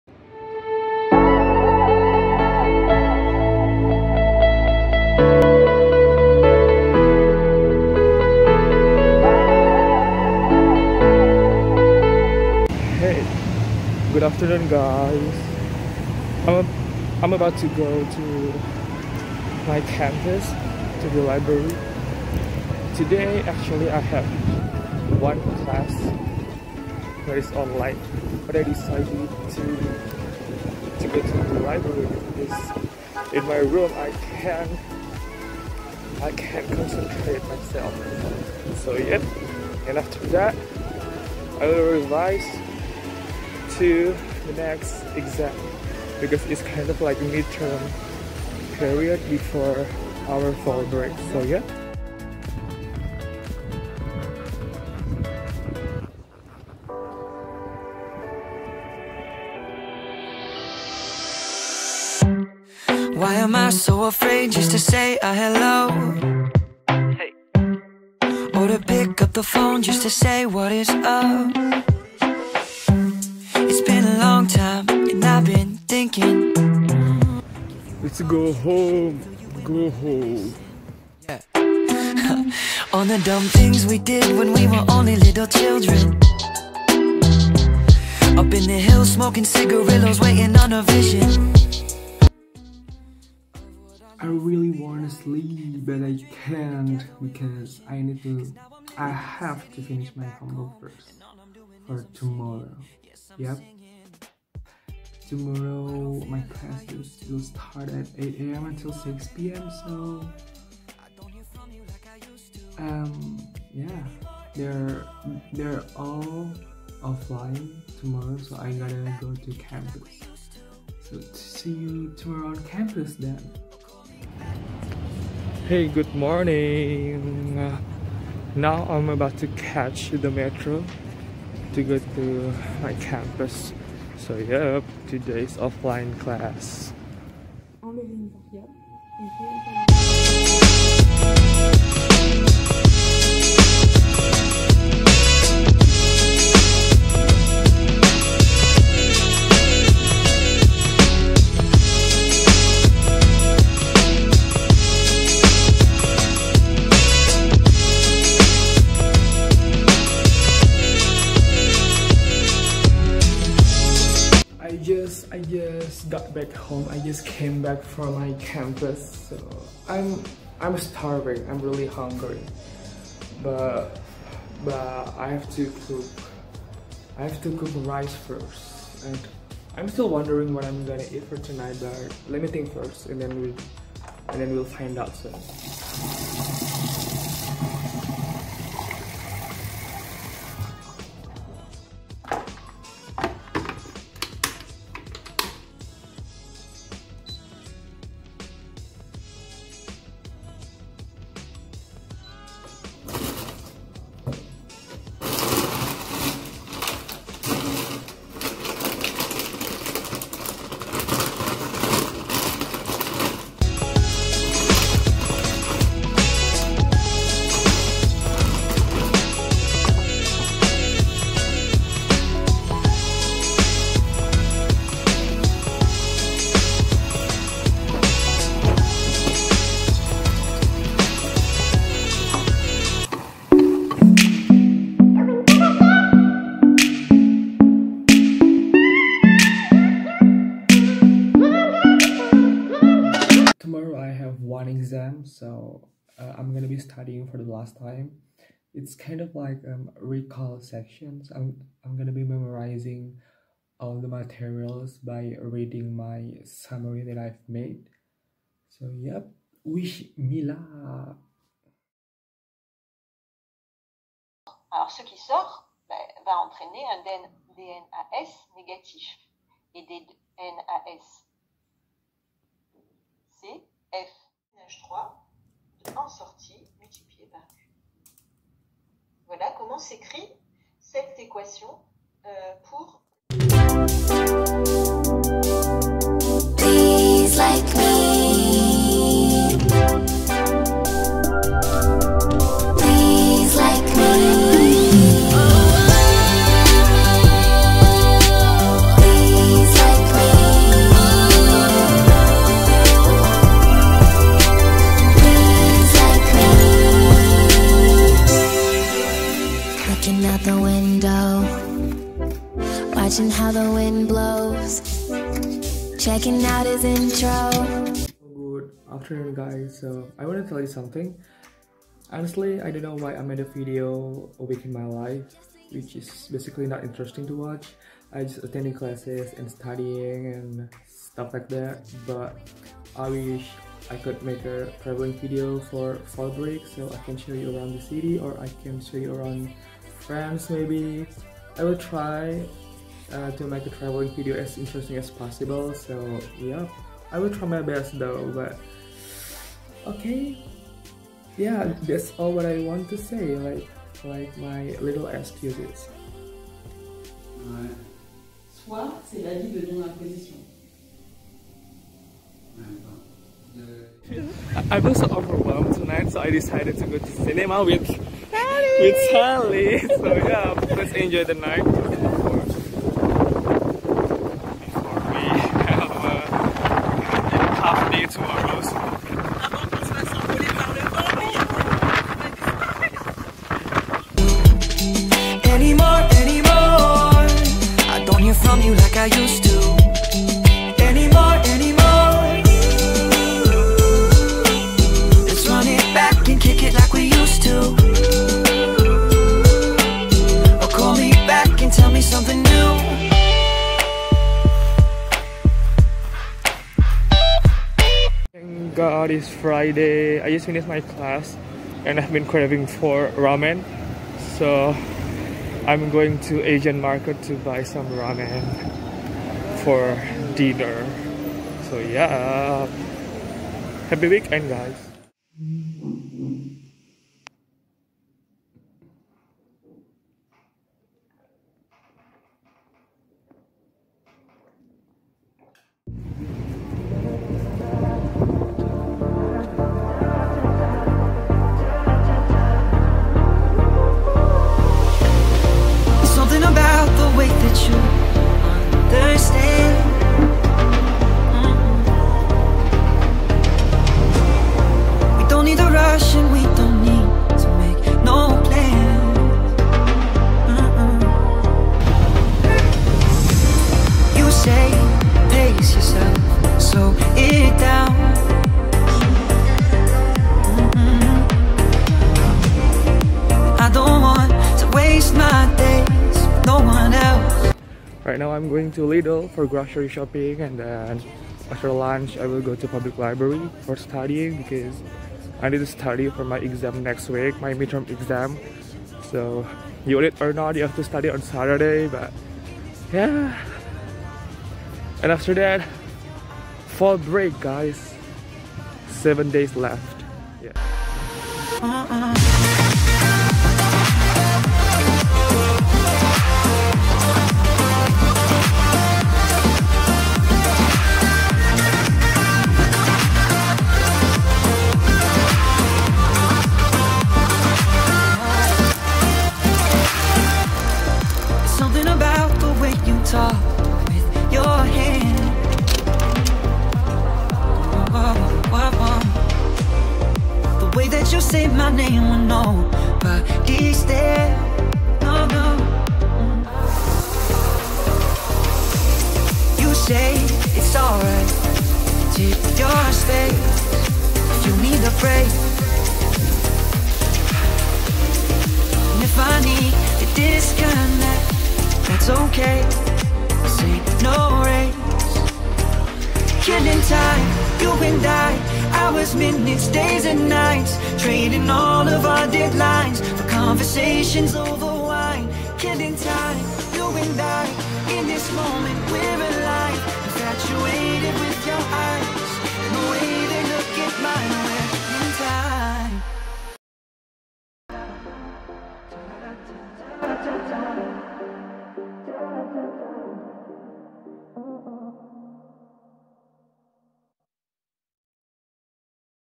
A WEEK IN MY LIFE (midterm exam, groceries shopping, normal life just boring :D)
Hi guys, I'm Firza and I'm Indonesian, currently studying chemical engineering at Aix-Marseille University, Marseille, France. On this occasion, I'm gonna be sharing the video a week in my life which is basically just attending classes and studying *boring* but still I wanna share it with you guys :).